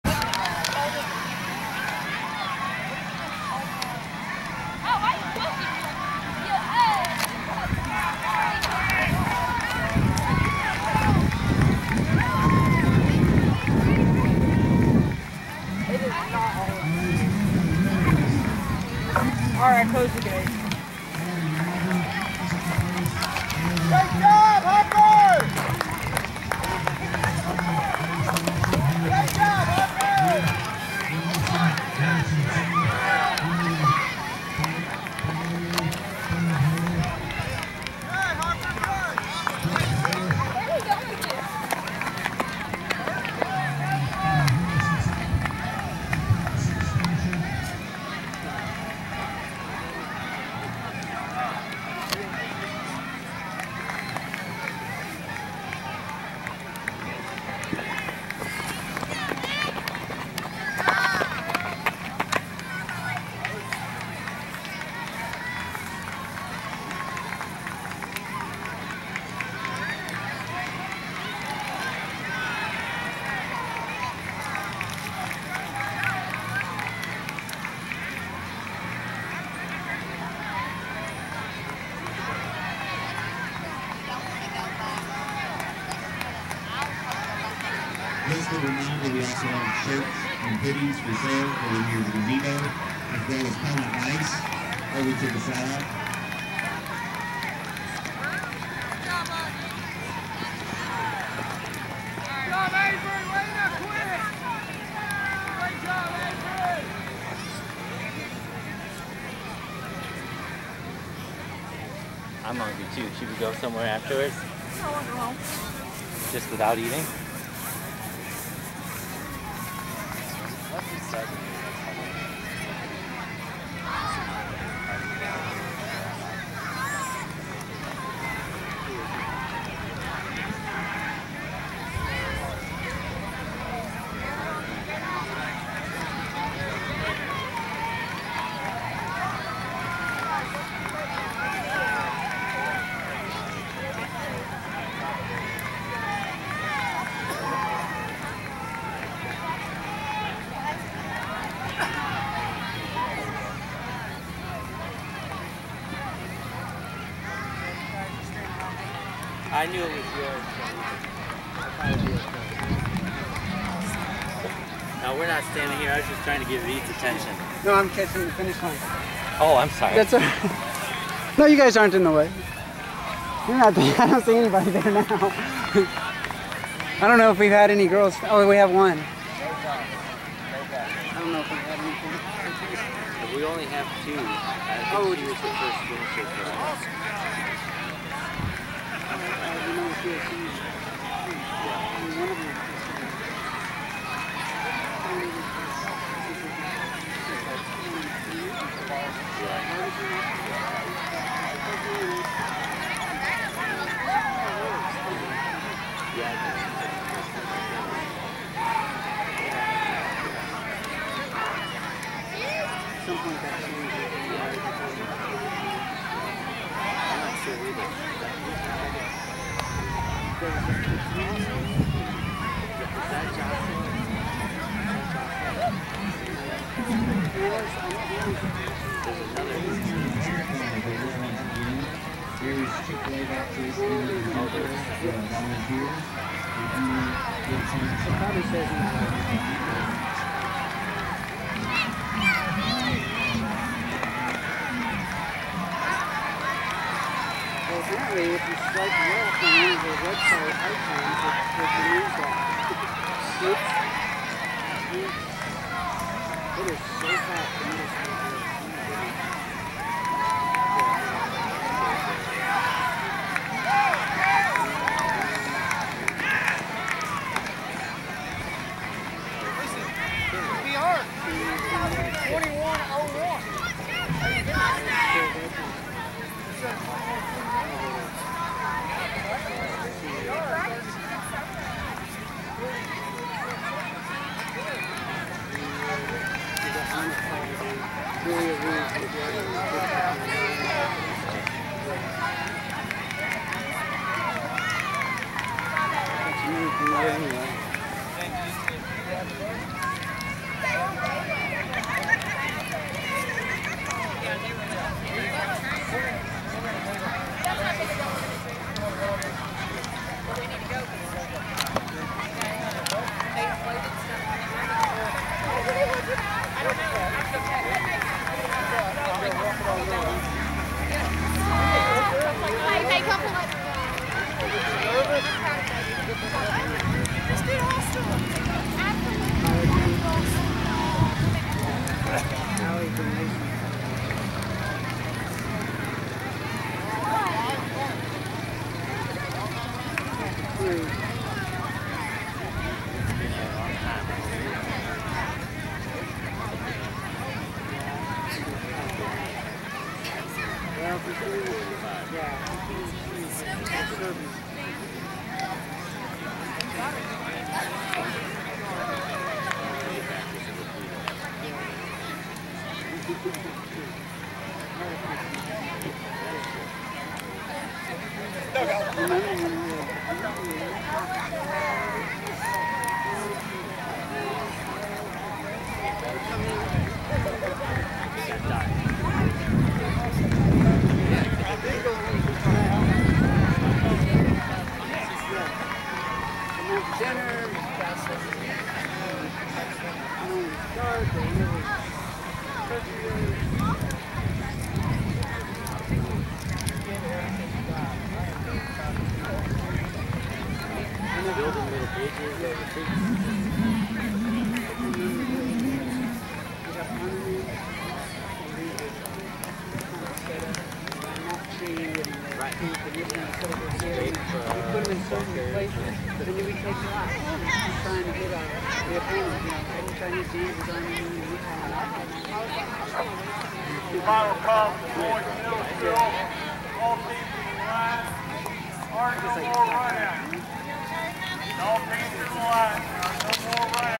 20241005 Daviess Classic MS Girls 4K Partial Finish
October 5, 2024 Daviess County Classic Cross Country meet at Yellow Creek Park in Owensboro, KY hosted by Daviess County HS.